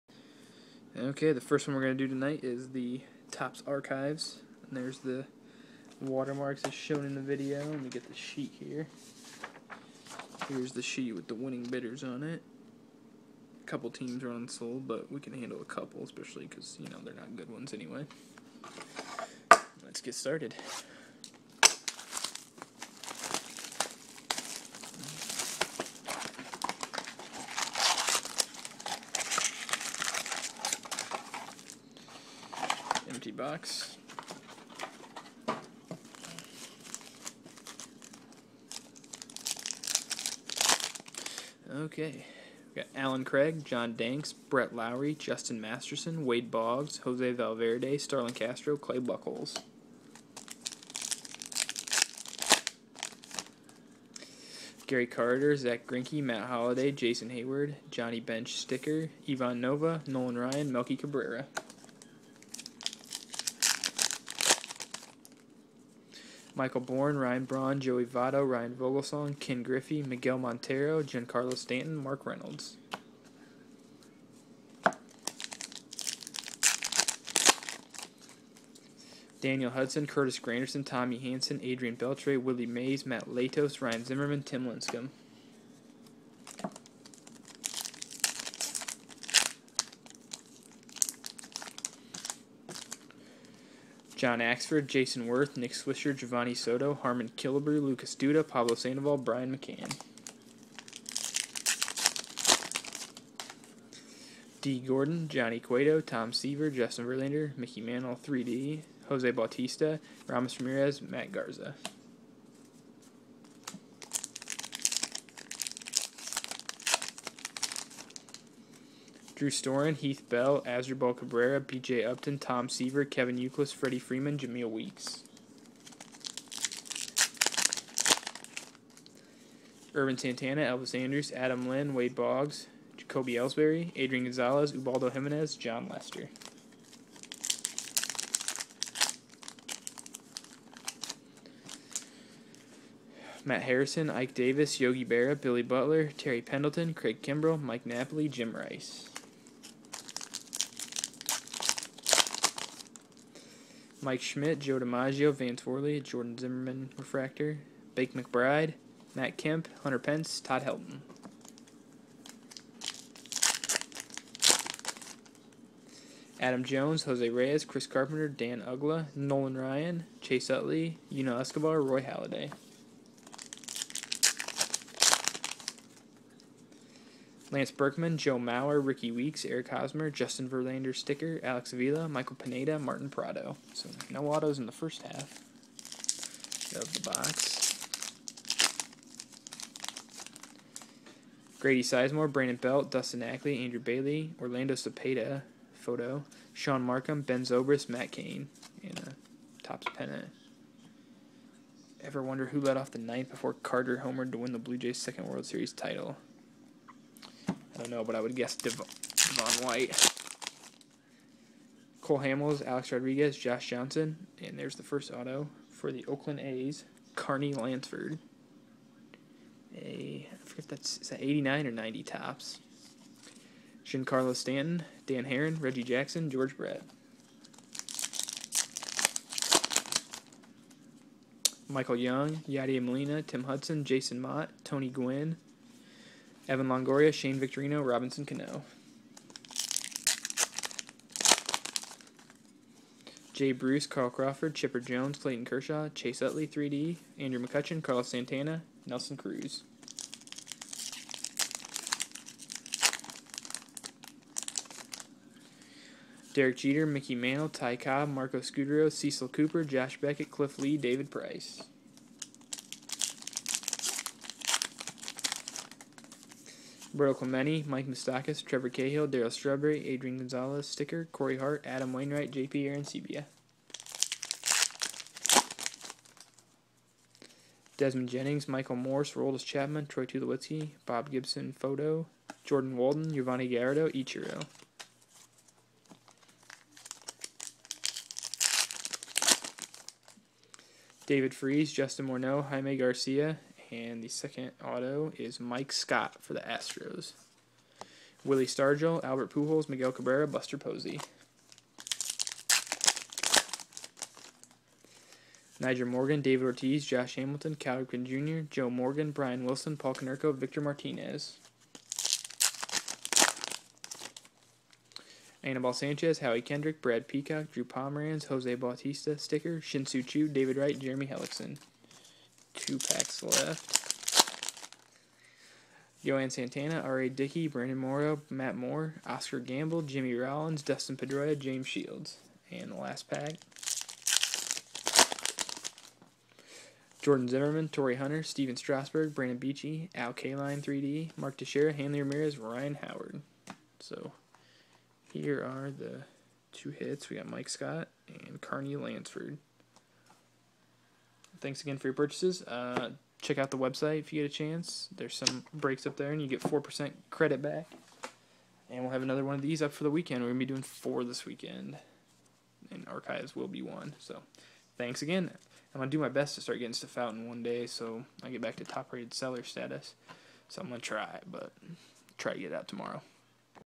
2012 Topps Archives Box Break #6
2012 Topps Archives Box Break for ebay auctions ended on 6/27

Thanks for your purchase and don't forget to check out for more great breaks!

The next Topps Archives Box Break will go up Thursday 6/28 and end on Sunday.